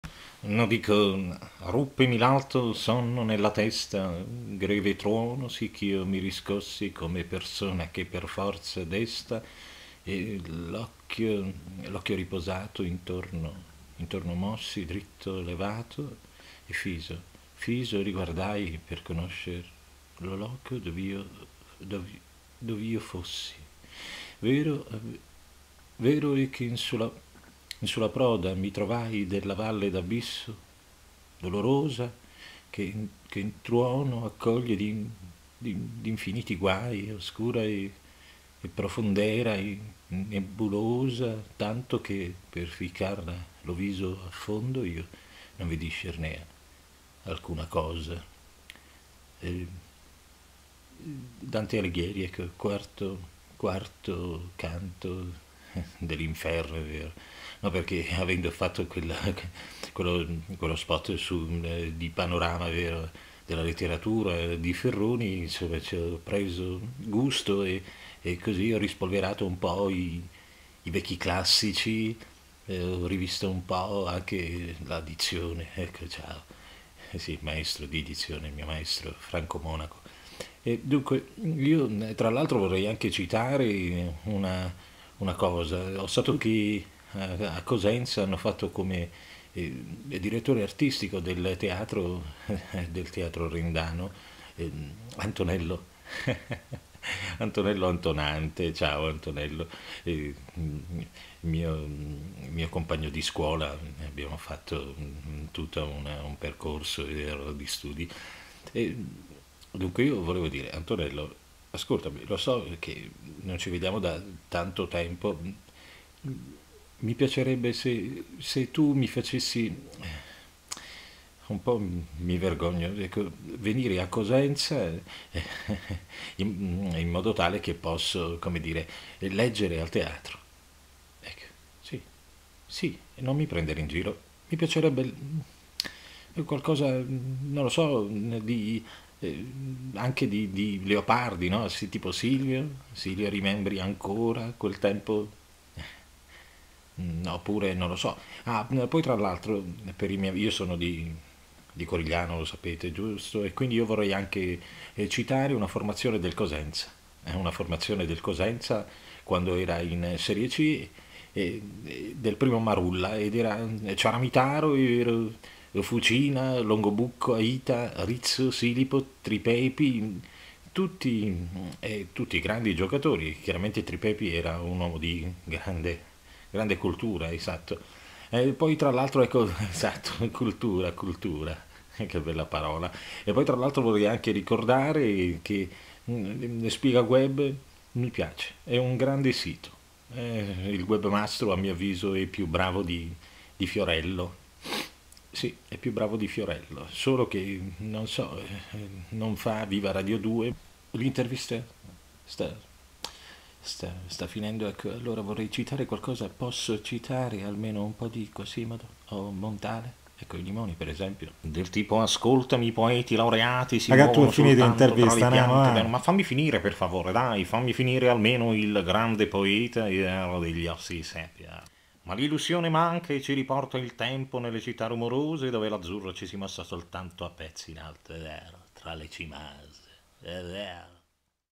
Ringhio Gattuso legge la Divina Commedia
Gattuso che legge il quarto canto è plausibile? E' possibile un riavvicinamento agli anni dei suoi studi classici? Quando verrà il momento dell'Eneide? Il sito Spigaweb.org ancora una volta ci sorprende con un'esclusiva. Dopo lo Spot, Gattuso è più "letterato" che PRIA!!!